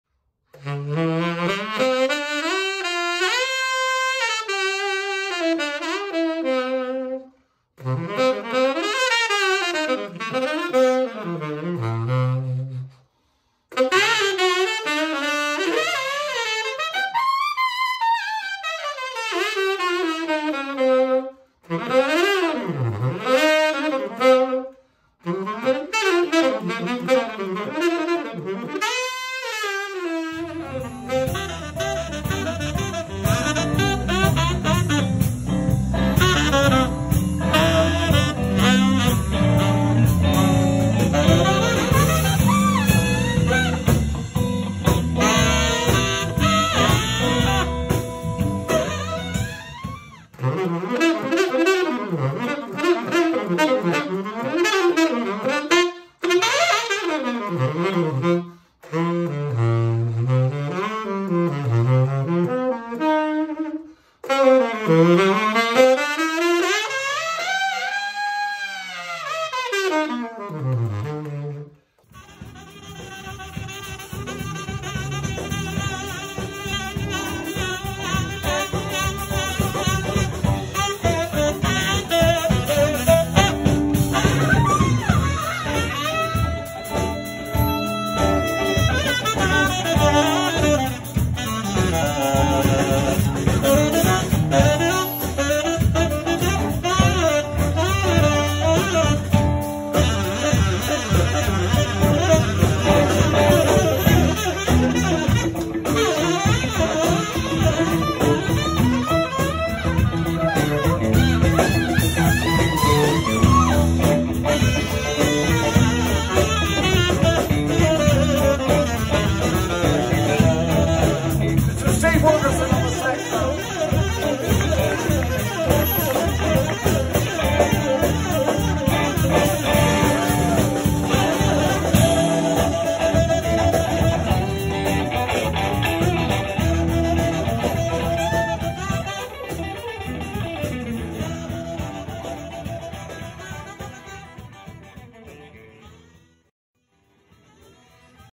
SUGAL STEALTH MOUTHPIECES -DEMOED by STEVE WILKERSON
Gary Sugal,  master artisan and skilled model maker of saxophone mouthpieces, renowned also for his extensive experience in jewelry manufacturing and CAD CAM techniques, has created three new mouthpiece models for tenor saxophone.

His inspiration for designing these models came from watching a recent episode of the TV show "60 Minutes" about fighter jets in the  US Air Force, which led him to create the "Stealth" saxophone mouthpiece series

Using state-of-the-art CAD/CAM design software and a variety of in-house 3D printer machining processes, Sugal crafted three powerful mouthpieces that make up the "Stealth" series: the STEALTH-GS, the STEALTH-MB-VI, and the STEALTH-KW-VI.

These models all produce a bright and fat cutting sound, accompanied by an incredible edge that sounds amazing.
Sugal's design has taken mouthpiece manufacturing to new heights by incorporating his tried-and-true innovations that capture the elusive nuances that mass-produced mouthpieces from other manufacturers lack.